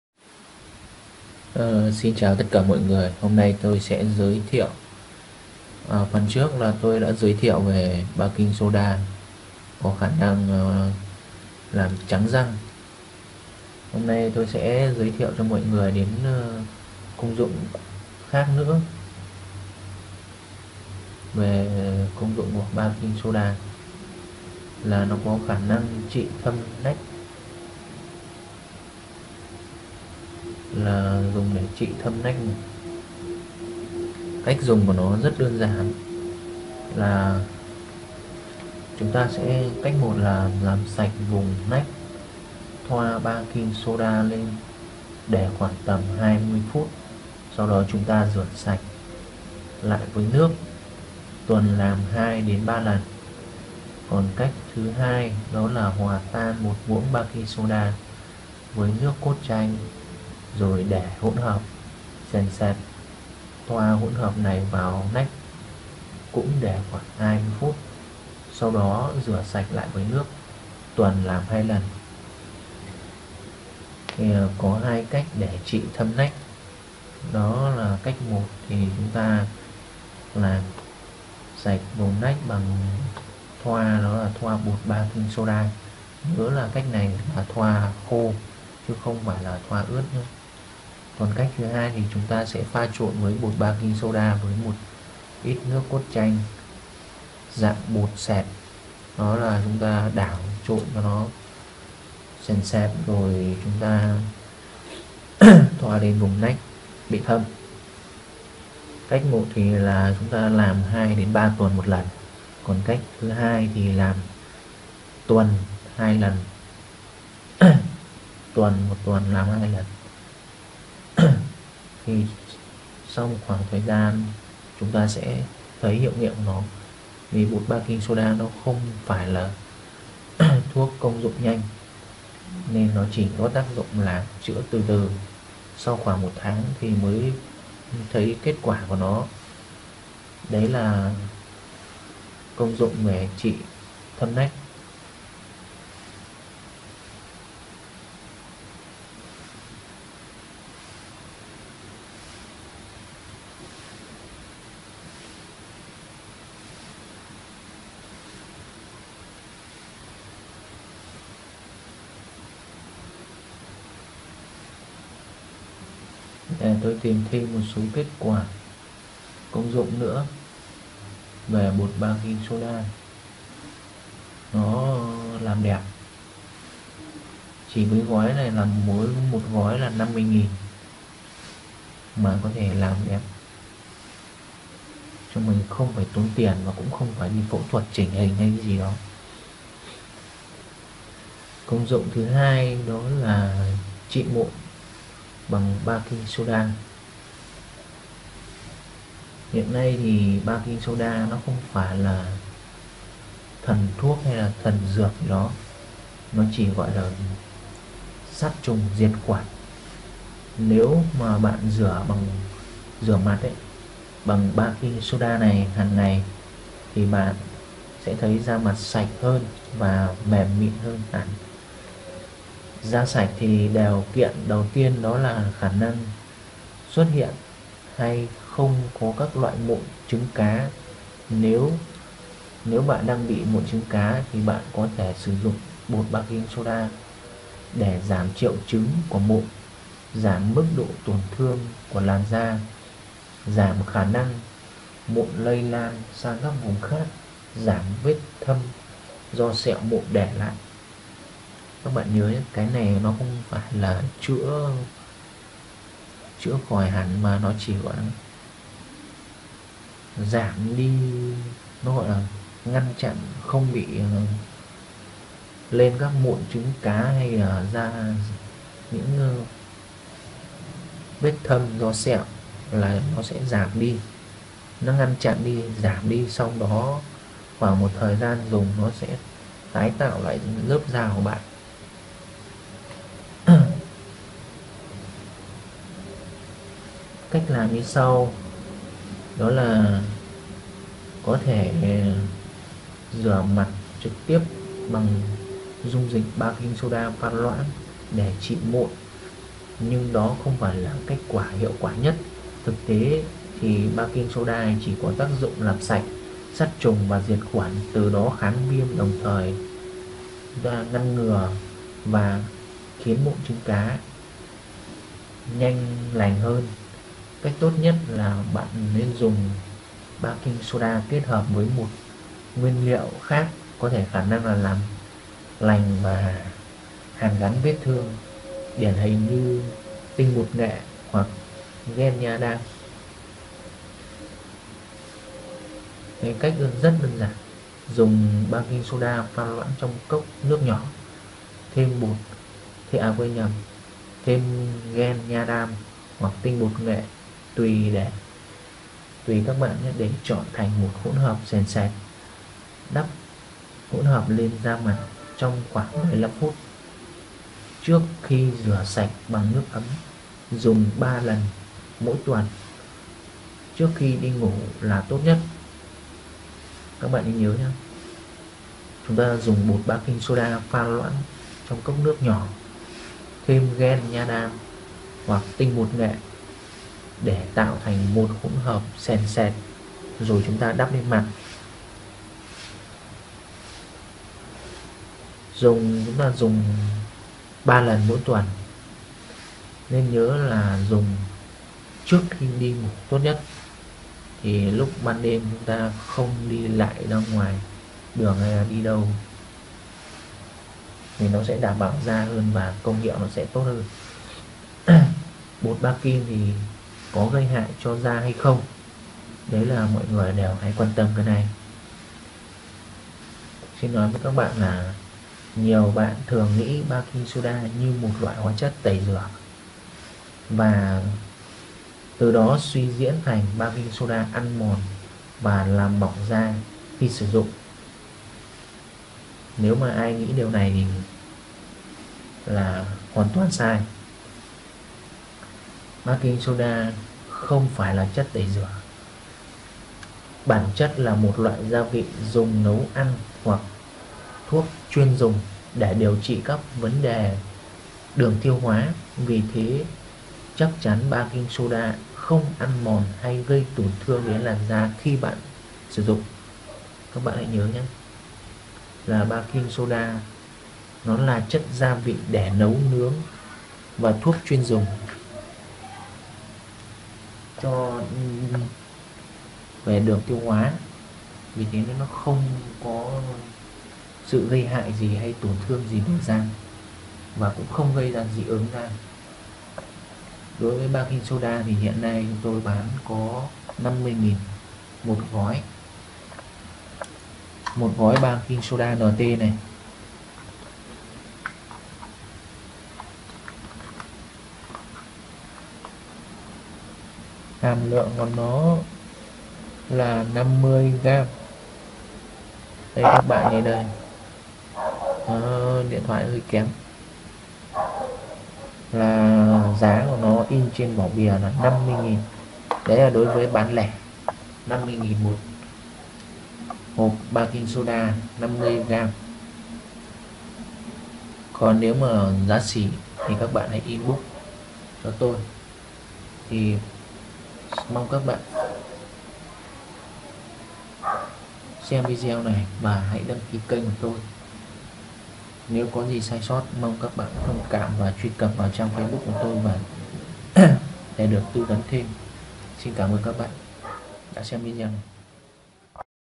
cách Trị Thâm Mụn
Hướng dương;  dẫn  cách trị mụ thâm và đắp mặt nạ bằng baking soda giúp bạn làm đẹp chỉ với 50k,
trị thâm nách cùng Baking soda"
👓👓 Cách 1: làm sạch vùng nách, thoa baking soda để khoảng 20 phút 💨 rửa sạch lại với nước
Tuần làm 2_3 lần
👓👓 Cách 2: hòa tan 1 muỗng baking soda với nước cốt chanh để có hỗn hợp sền sệt, thoa hỗn hợp này vào nách, để 20 phút 💨 rửa sạch lại với nước
Tuần làm 2 lần
💤💤💤💤💤💤💢💥💥💥💥💥 Còn 27 công dụng nữa ib mình tư vấn thêm nhé
Giá lẻ chỉ 50 k/ hộp 50 gam